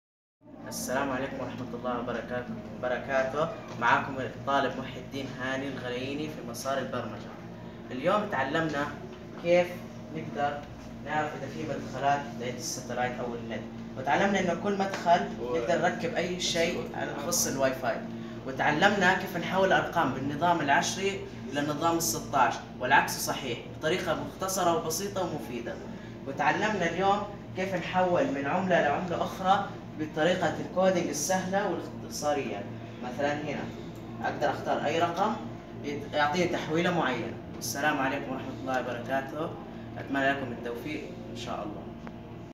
مسار (برمجة الحاسوب)
أحد طلاب مسار (برمجة الحاسوب) يتحدث عن إنجازات طلاب المسار وما تعلموه، وانطباعهم عن العمل بالبرنامج.